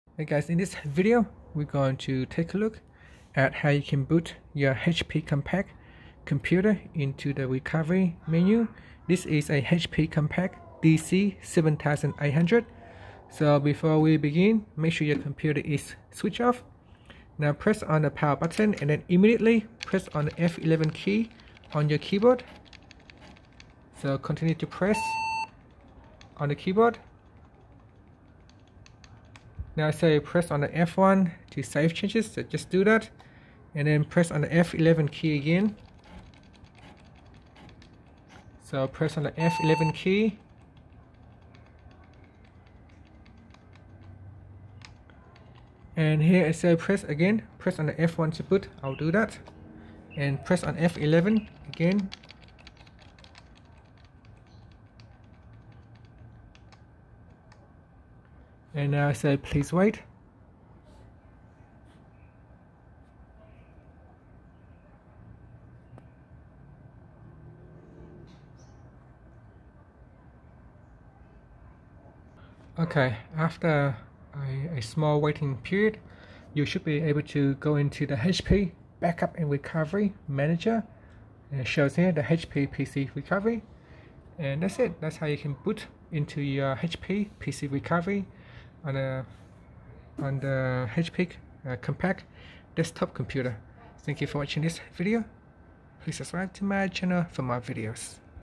HP Compaq DC7800: How to Boot Into PC Recovery Menu/Partition
Learn how you can boot into PC recovery menu/partition on the HP Compaq DC7800.